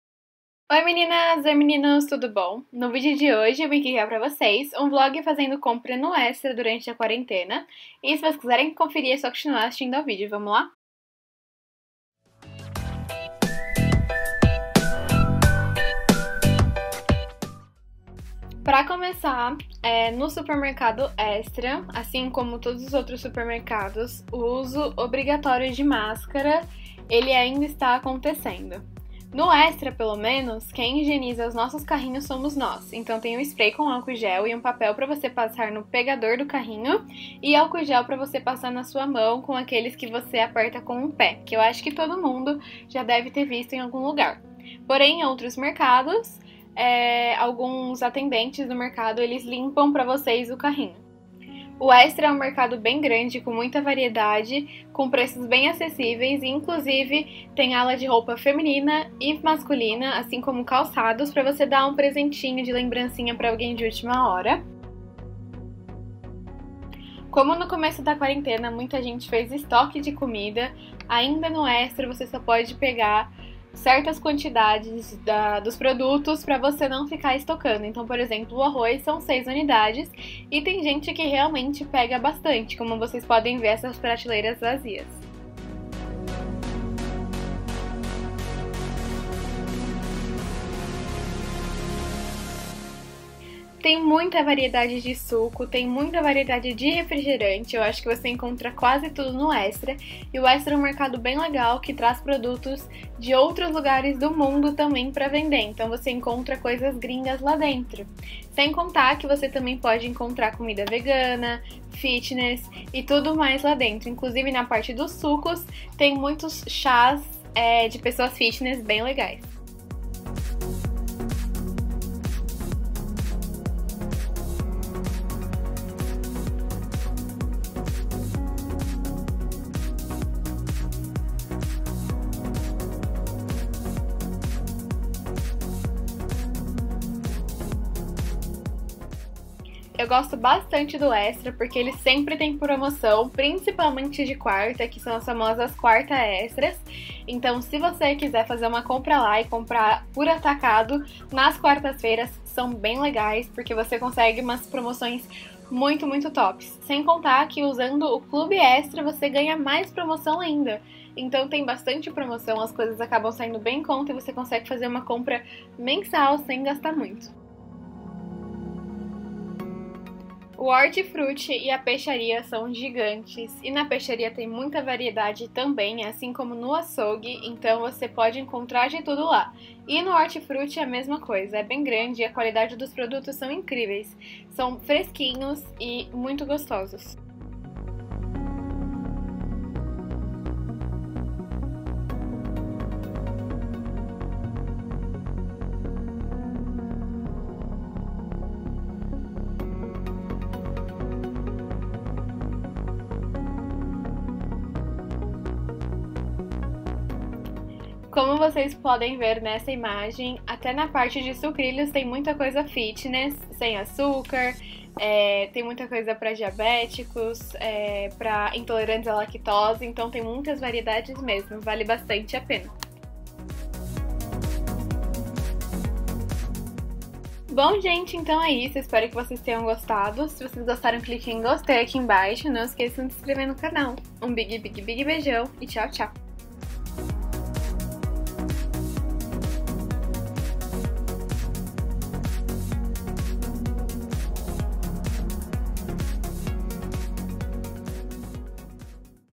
VLOG NO EXTRA: Fazendo compras durante a quarentena
Oi meninas e meninos, tudo bom?
Hoje eu trouxe mais um vídeo de coração pra vocês. Vamos conferir?

Um big big beijão e tchau, tchau!